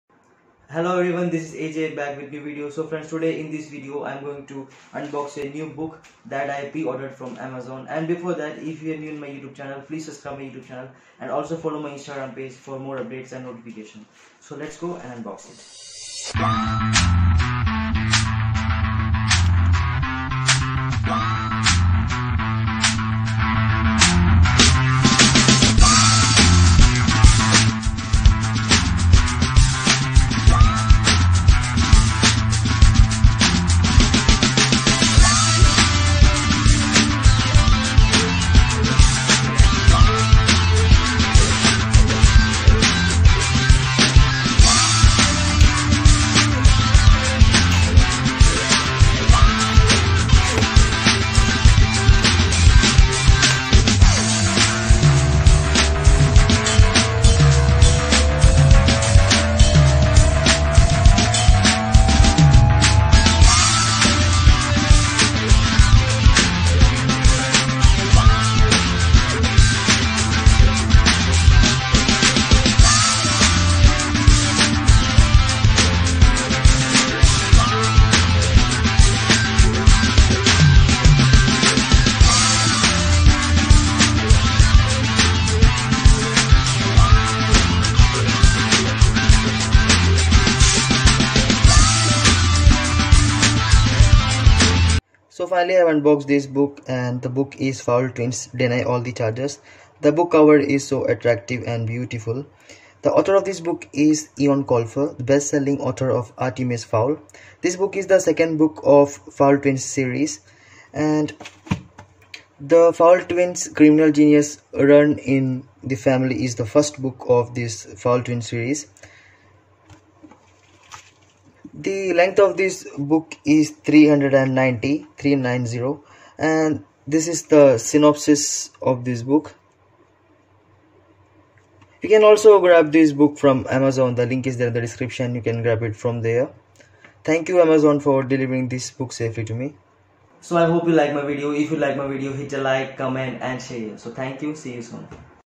Fowl Twins:Deny All Charges|by Eion Colfer|Book 2|Unboxing Book|Amazon|Pre-order
About Book
The second book in the blockbusting new series from the bestselling author of Artemis Fowl.

The second Fowl Twins adventure starts with a bang – literally.

Artemis's little brothers Myles and Beckett borrow the Fowl jet without permission, and it ends up as a fireball over Florida. The twins plus their fairy minder, the pixie-elf hybrid Lazuli Heitz, are lucky to escape with their lives.

The Fowl parents and fairy police force decide that enough is enough and the twins are placed under house arrest. But Myles has questions, like: who was tracking the Fowl jet? Why would someone want to blow them out of the sky? These questions must infuriate someone, because Myles is abducted and spirited away from his twin.

Now Beckett and Lazuli must collaborate to find Myles and rescue him – not easy when it was Myles who was the brains of the operation. Their chase will take them across continents, deep underground, and into subaquatic super villain lairs. They will be shot at, covered in spit, and at the receiving end of some quite nasty dwarf sarcasm. But will Beckett be able to come up with a genius plan without a genius on hand…?

Eoin Colfer worked as a teacher before turning his attention fo full-time writing for children. He is best known for his phenomenally successful Artemis Fowl series. Eoin lives with his family in Ireland.